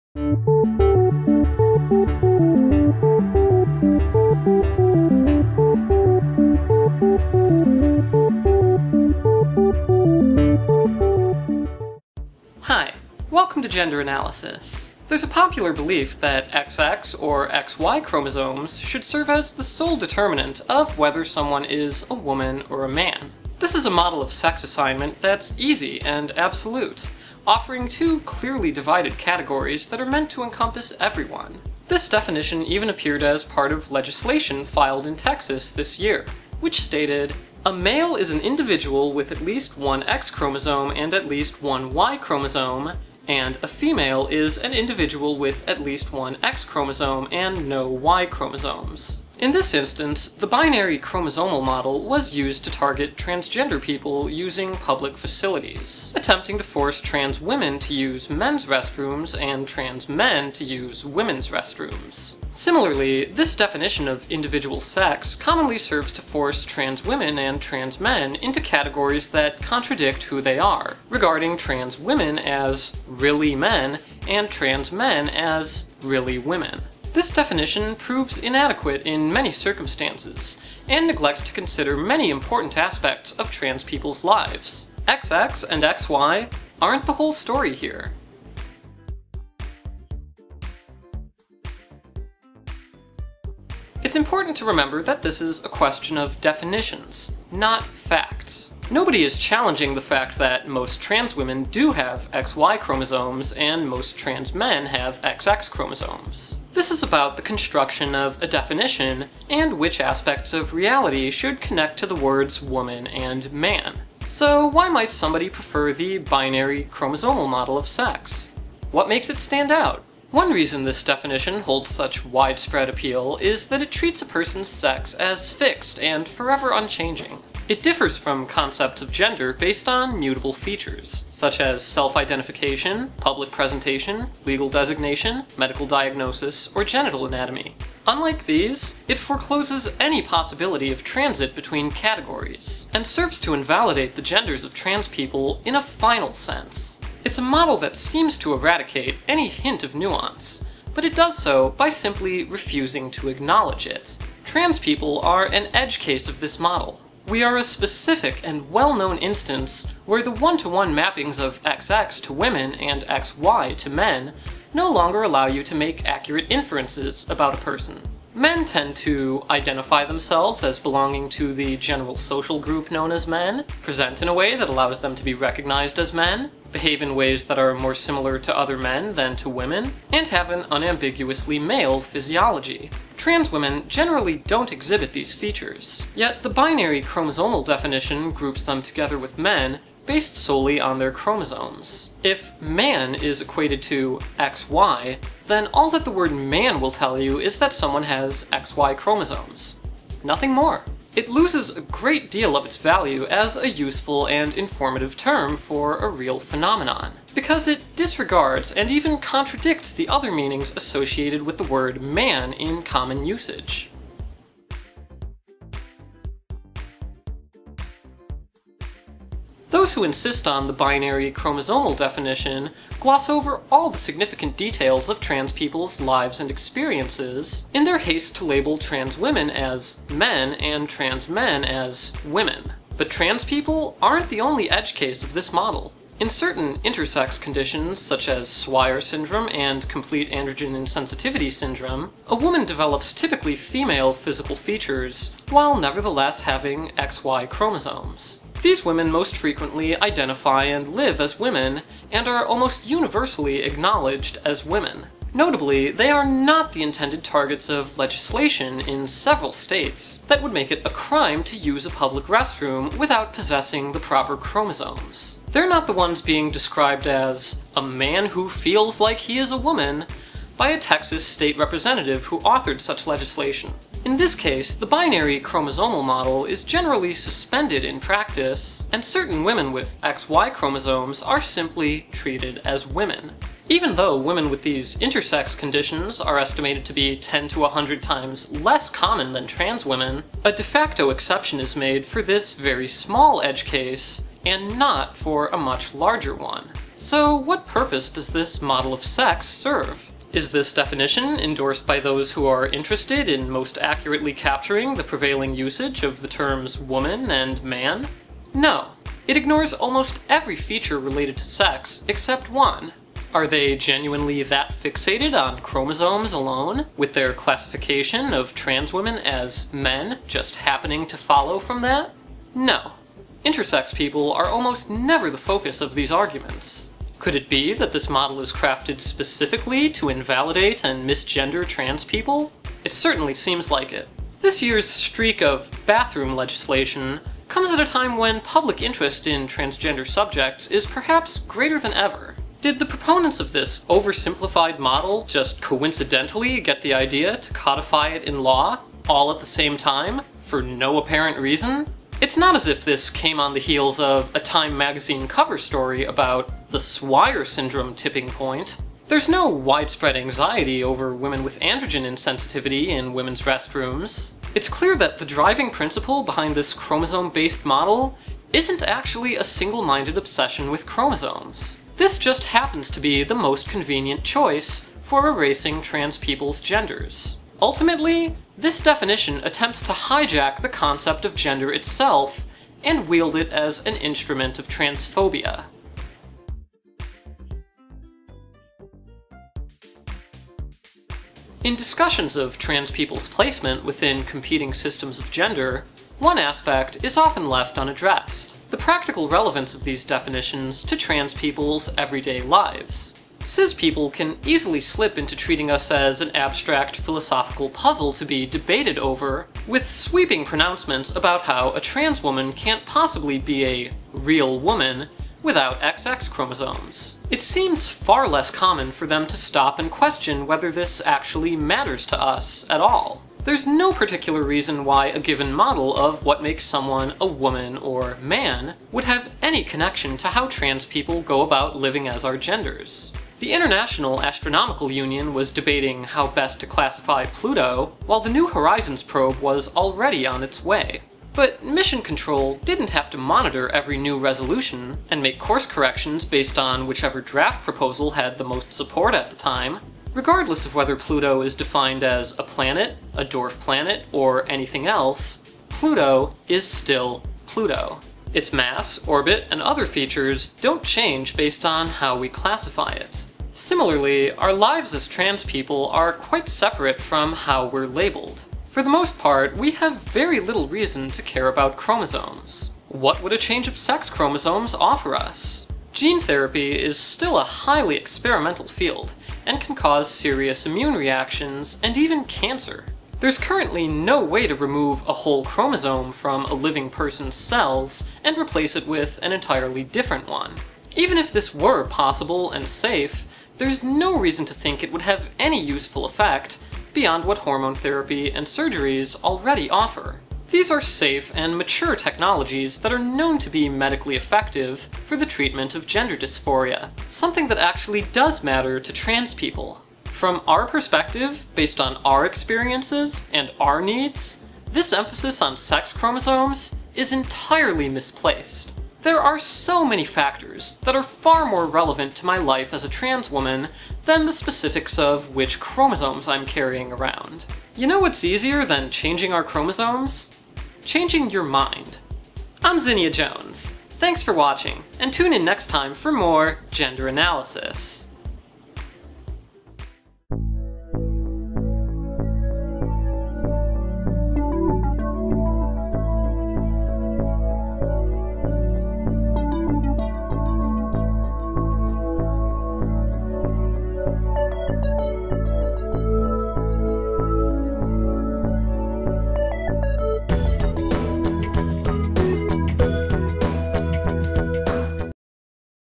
Gender Analysis 14. Chromosomes: cis expectations vs. trans reality
Why XX and XY chromosomes don't mean what you think they do.